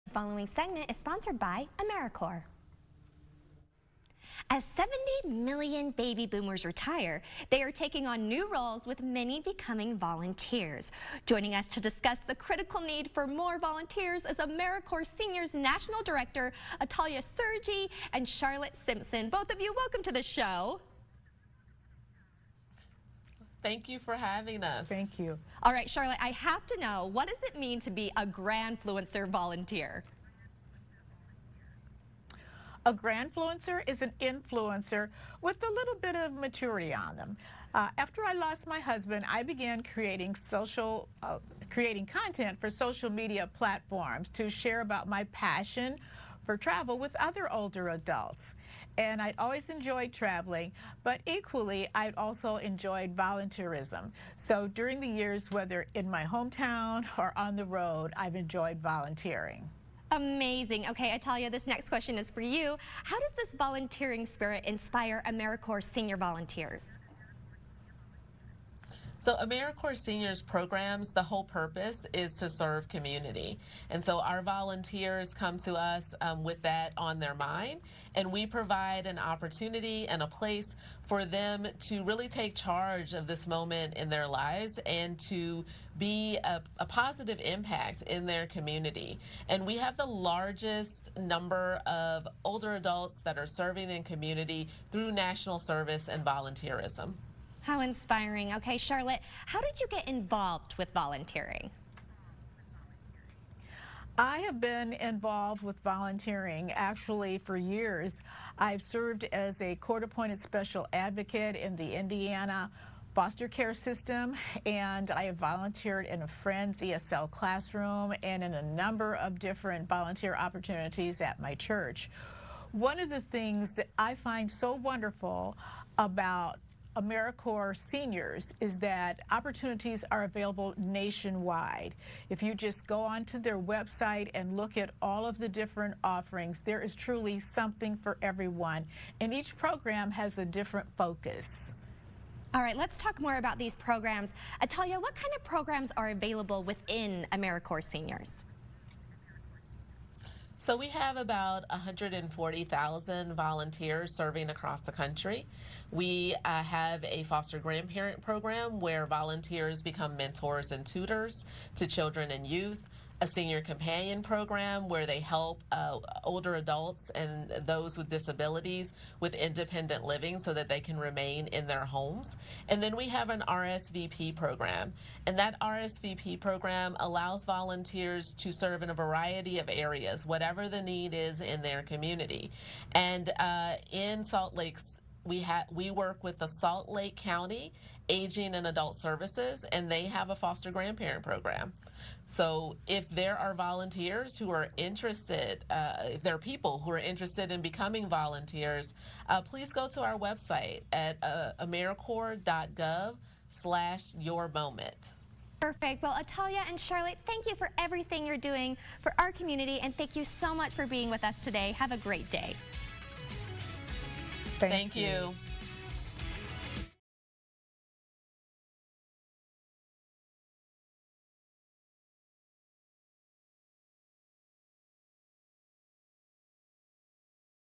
AmeriCorps seniors volunteer programs
Atalaya Sergi and Charlotte Simpson from AmeriCorps discussed the importance of volunteering with Elora.